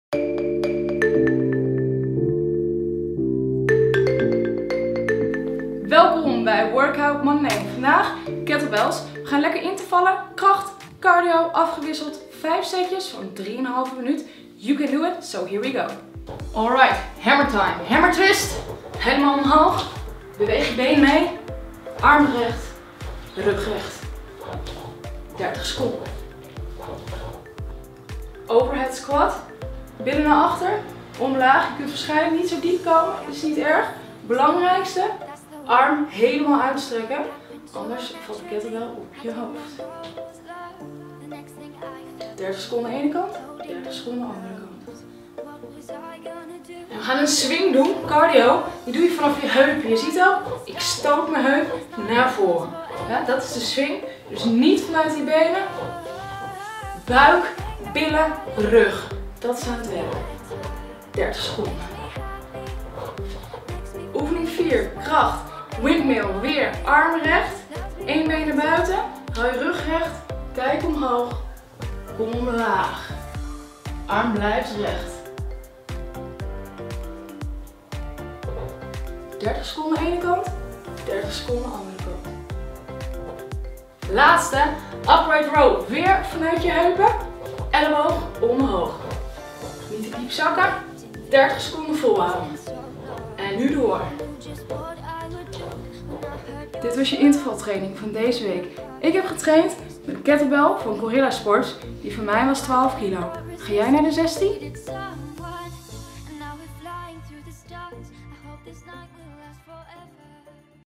Workout Monday: Kettlebell training
Workout Monday: thuis trainen met een Kettlebell!

Mayke laat je 5 van haar favoriete Kettlebell oefeningen zien!

Ook thuis trainen? Check:  Gorilla Sports NL​